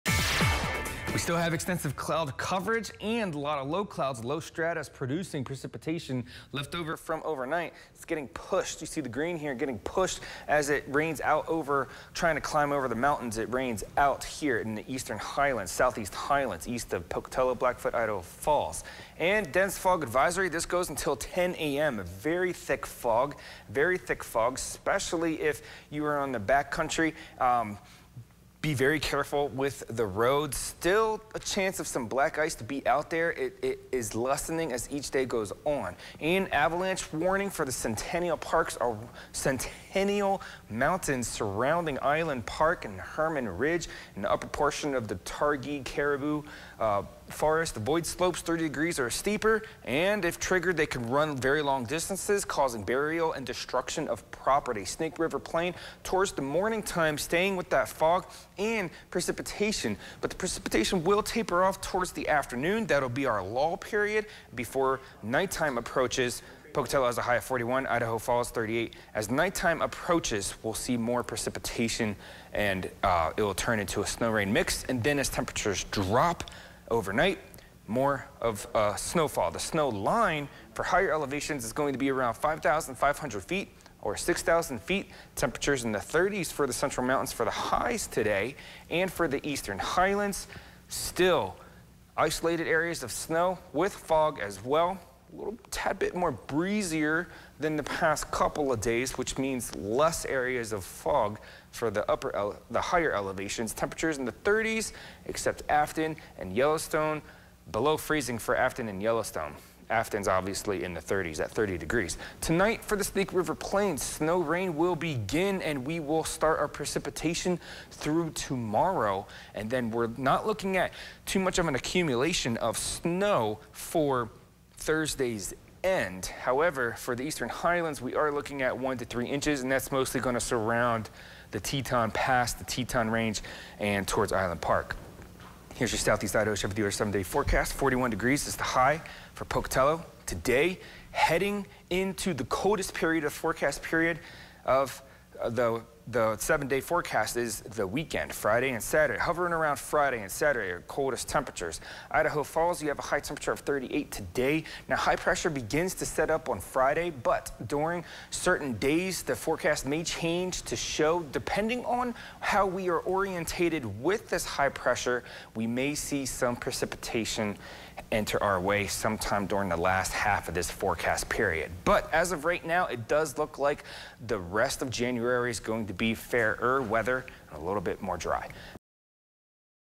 Eric's Storm Tracker Weather for Wed Jan 24
A morning dense fog warning. Precipitation tapers off then on again tonight with a snow-rain mix into tomorrow.